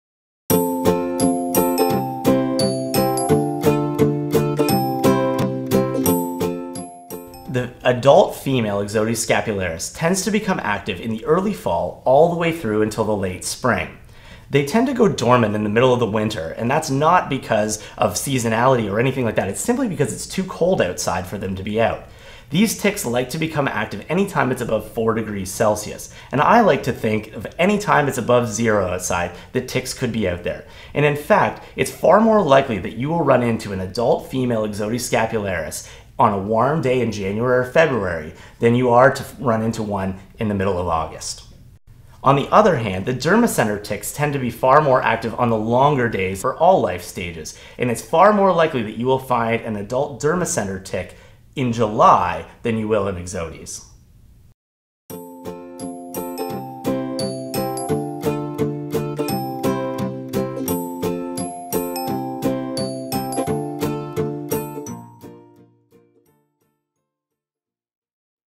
Question 12 : Quand les tiques s'activent-elles?
Une série de 12 vidéos intitulée «Que savez-vous vraiment au sujet des tiques?» (à partager), où le vétérinaire canadien Dr Scott Stevenson, B.M.Sc., M.Sc., D.M.V., répond aux questions fréquemment posées par les propriétaires d’animaux.